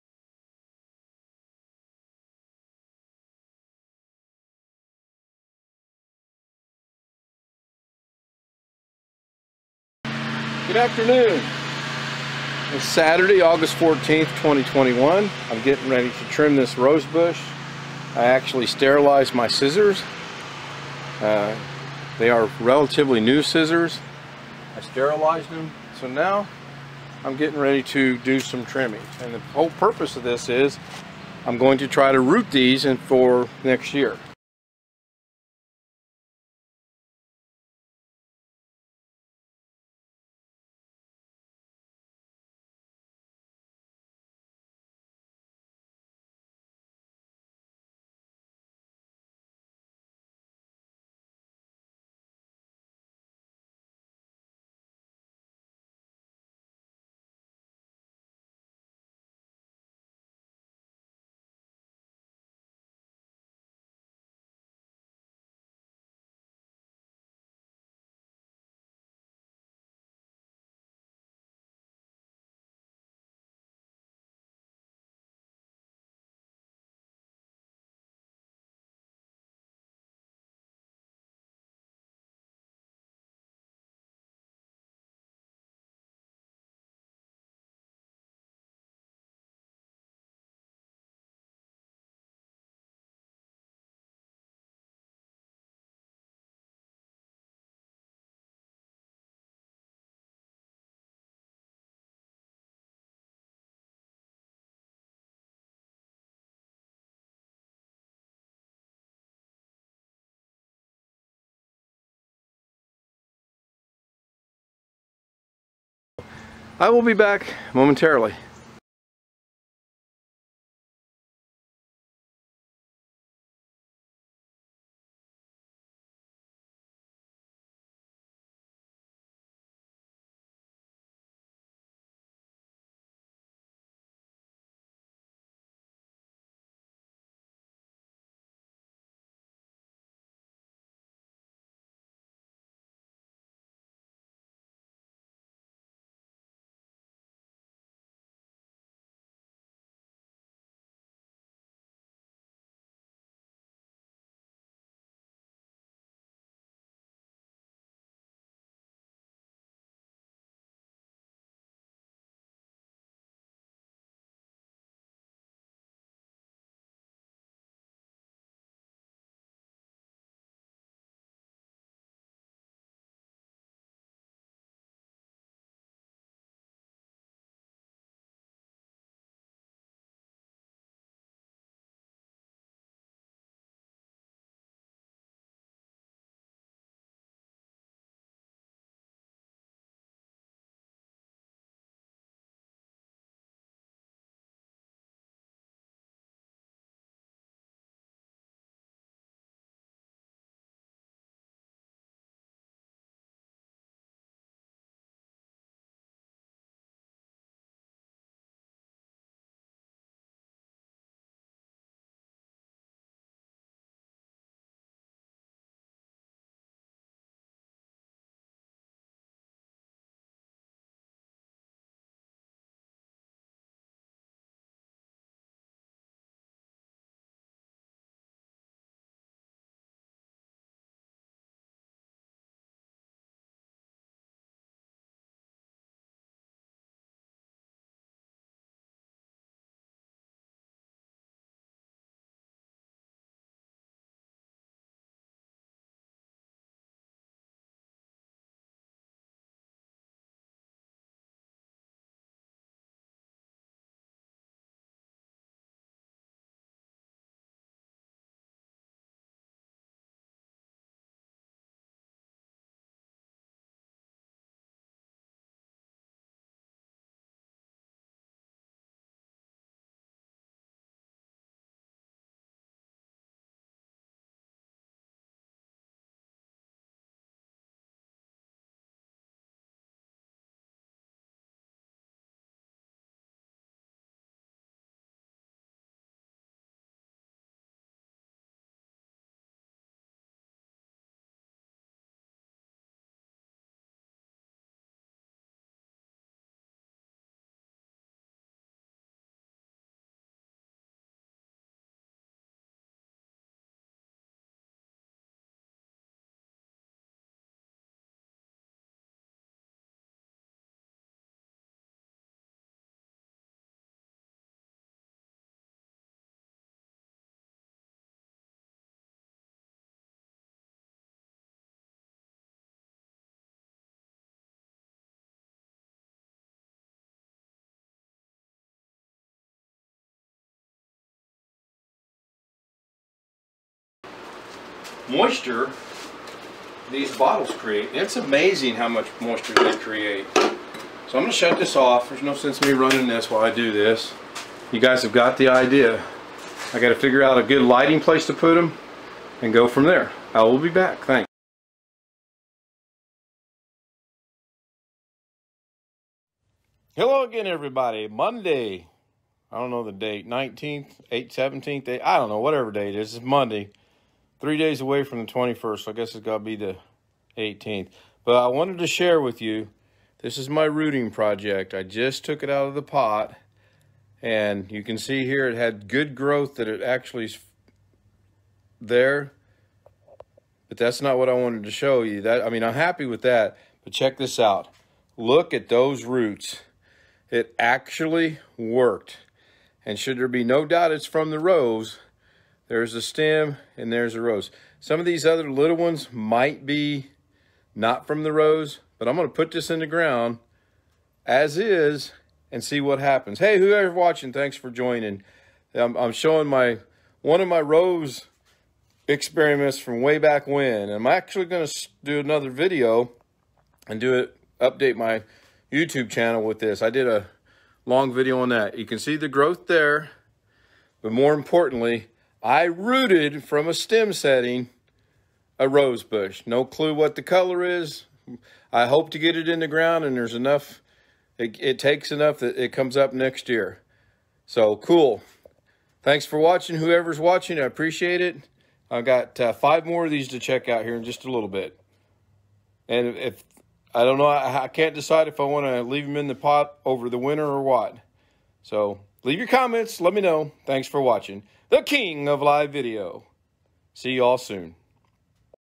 Rooting Roses Update:
Yesterday I checked on one of the rooting roses project I had started in August.  I did this on Facebook live so the video isn't the best.  Additionally I included the original stream but I fast fowarded it  800 times for obvious reasons.
Original stream was in August 2021
Thanks!